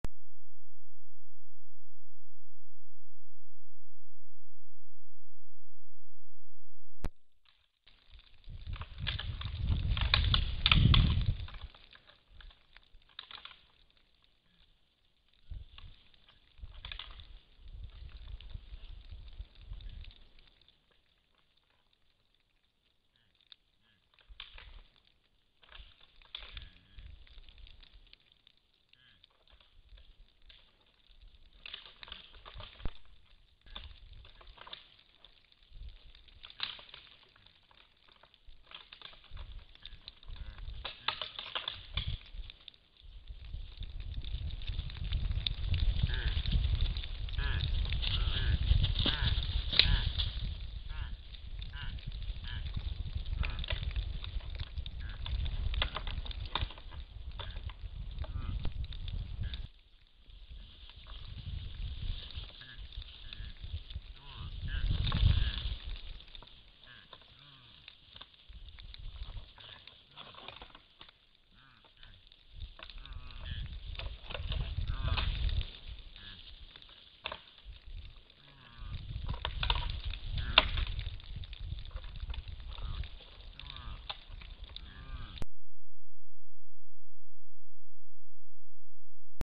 Zimowy sparing jeleni / Winter red deer sparring
Nagrań dokonano za pomocą wideopułapek w obszarze Natura 2000 "Beskid Żywiecki" w lutym 2014 r.
Video footages taken with camera traps in the Natura 2000 site "Beskid Żywiecki" in Polish part of Carpathian Mts, February 2014.